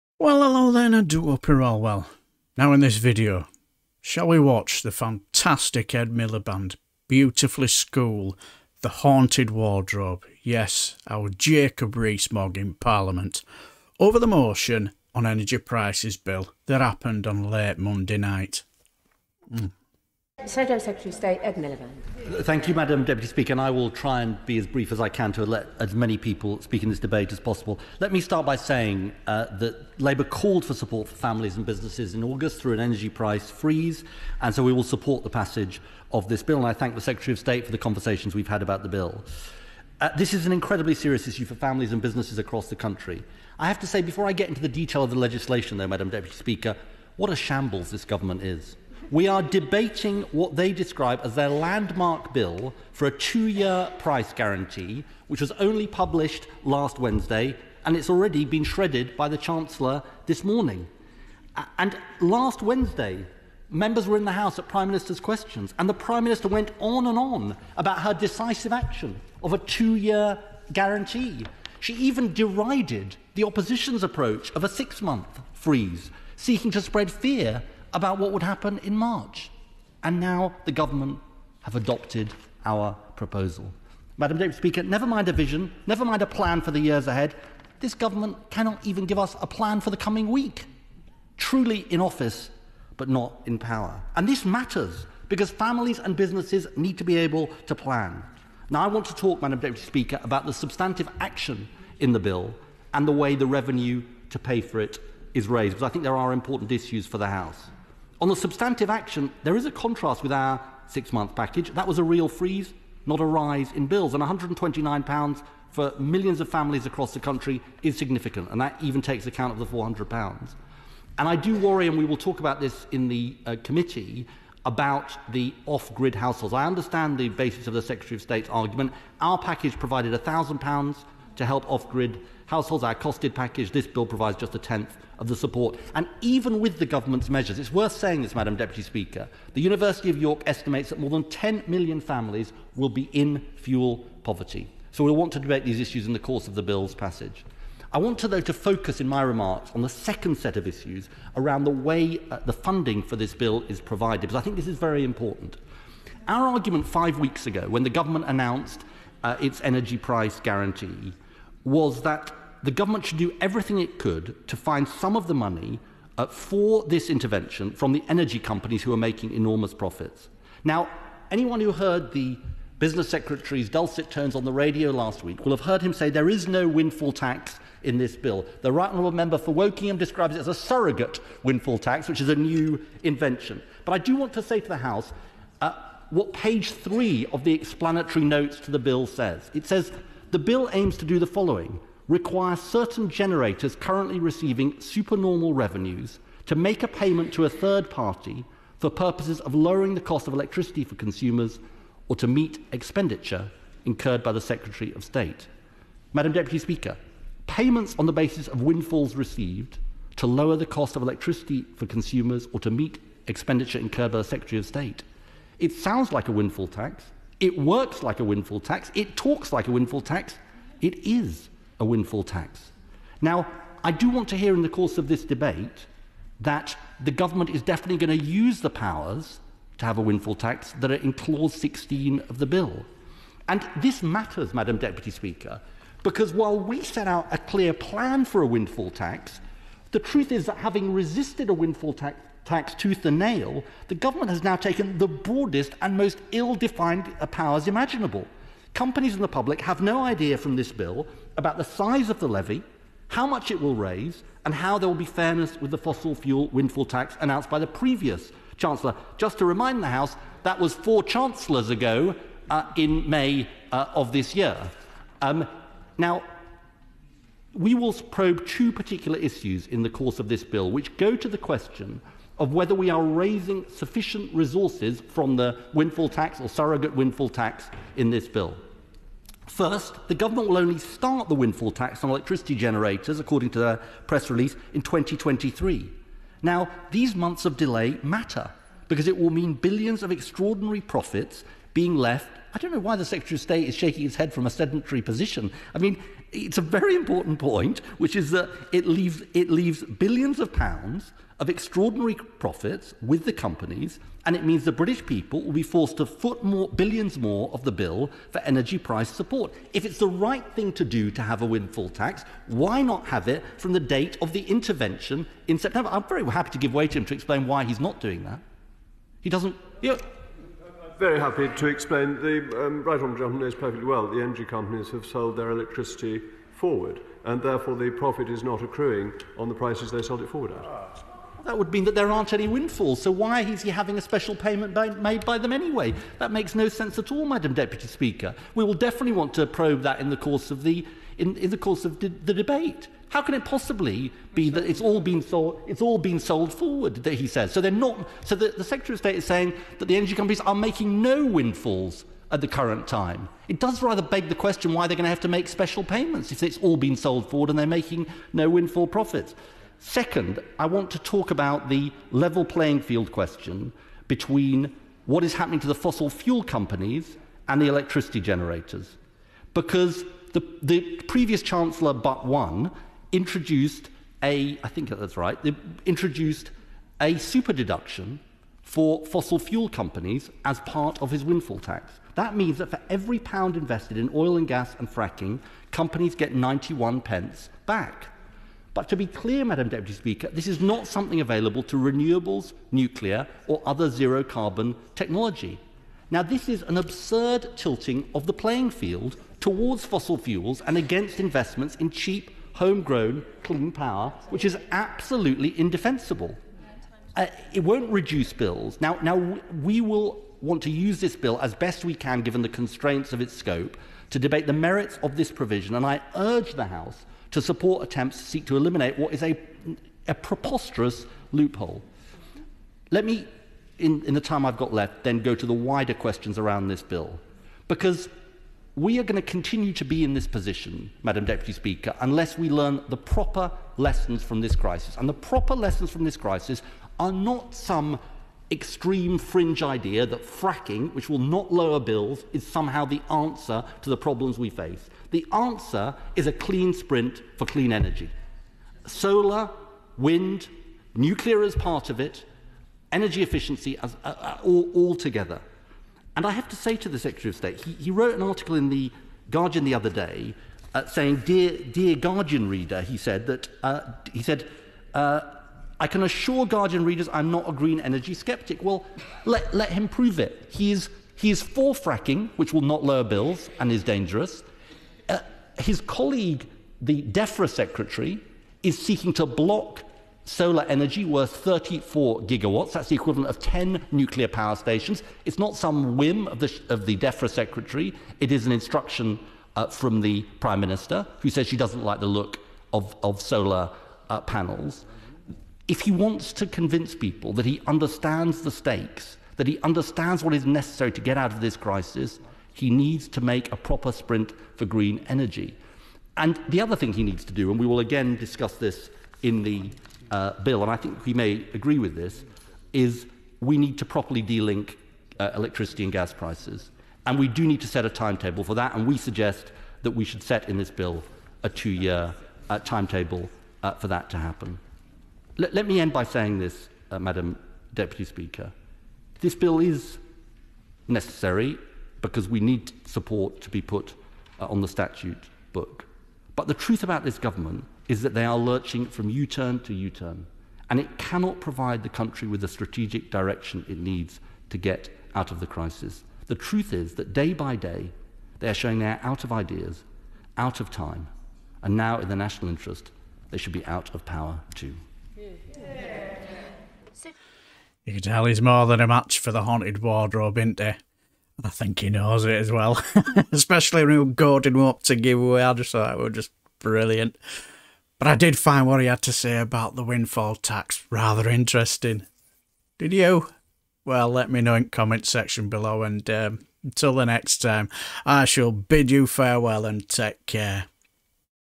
Fantastic Ed Miliband Beautifully Schools Jacob Rees-Mogg In Parliament?
0:00 Introduction
0:20 Fantastic Ed MiliBand, Beautifully Schools Jacob Rees-Mogg In Parliament?
9:17 Final Thought
Now in this video, shall we watch the fantastic Ed Miliband, beautifully school the haunted wardrobe Jacob Rees-Mogg, in parliament, over the motion on energy prices bill that happened late Monday evening?